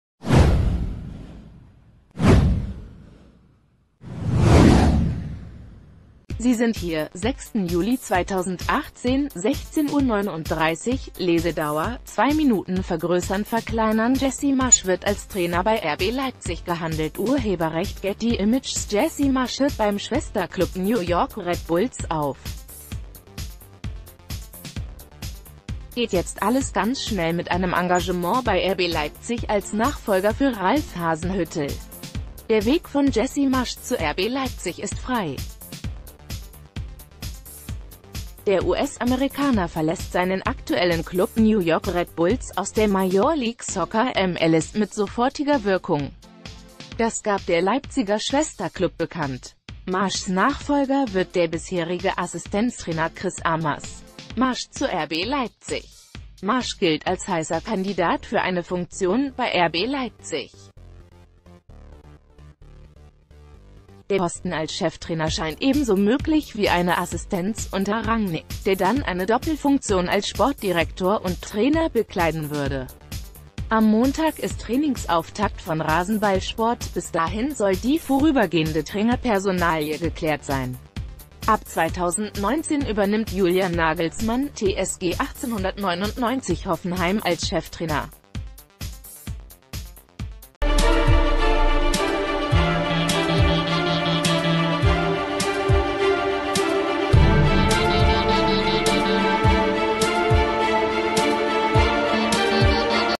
RB Leipzig: Jesse Marsch neuer Trainer? Abschied von New York Red Bulls
Sie sind hier: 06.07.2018 / 16:39 Uhr / Lesedauer: 2 Minuten vergrößernverkleinern Jesse Marsch wird als Trainer bei RB Leipzig gehandelt © Getty Images Jesse Marsch hört beim Schwesterklub New York Red Bulls auf. Geht jetzt alles ganz schnell mit einem Engagement bei RB Leipzig als Nachfolger für Ralph Hasenhüttl? Der Weg von Jesse Marsch zu RB Leipzig ist frei. Der US-Amerikaner verlässt seinen aktuellen Klub New York Red Bulls aus der Major League Soccer (MLS) mit sofortiger Wirkung. Das gab ...